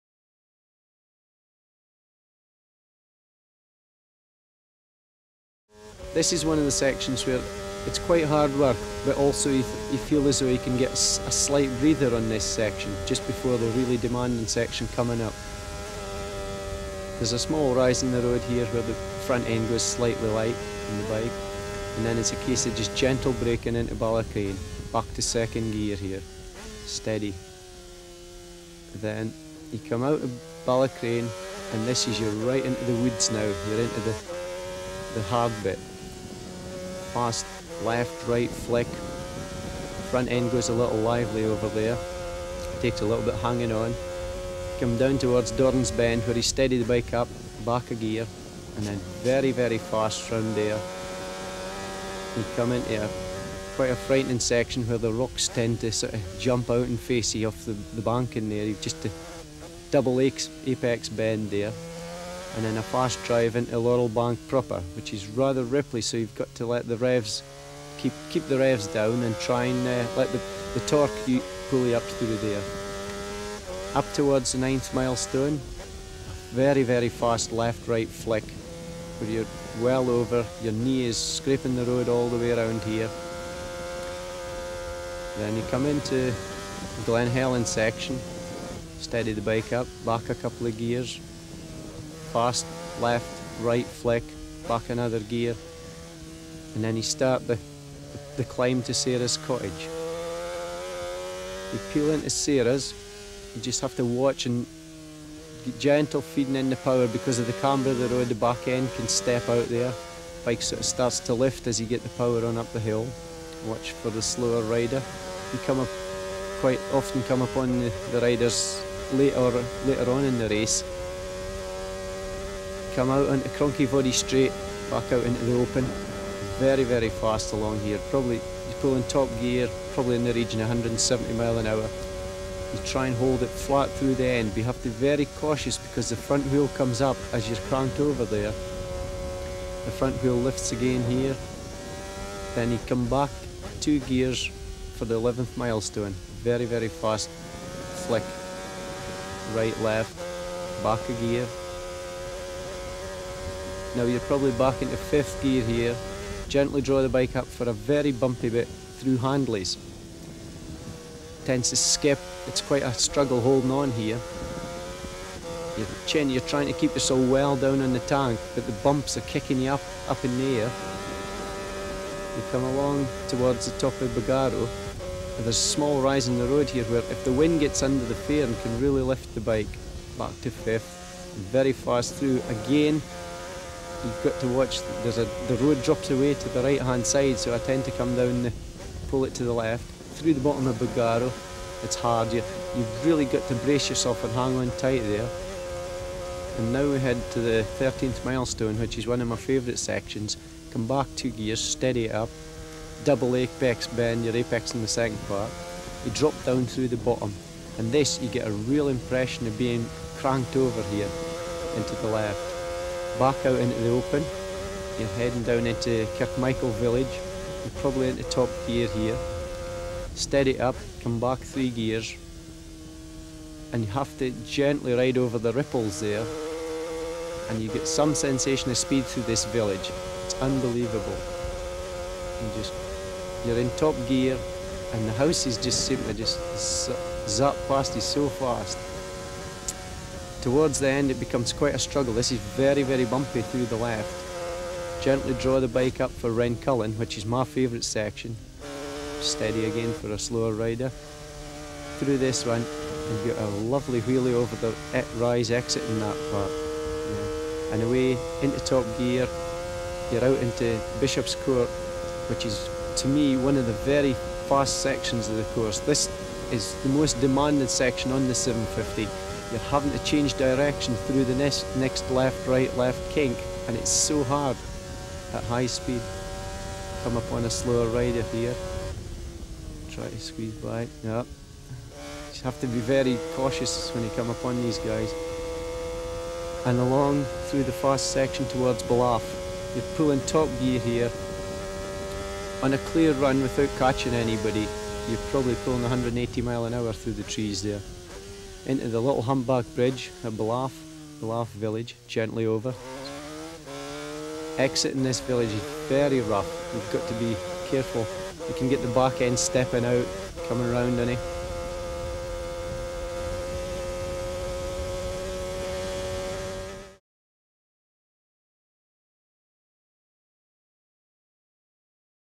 Steve Hislop - TT Flyer - Honda VFR 750
The Flying Scots man, Steve Hislop's on board footage from the 1990 TT, where he Averaged 120mph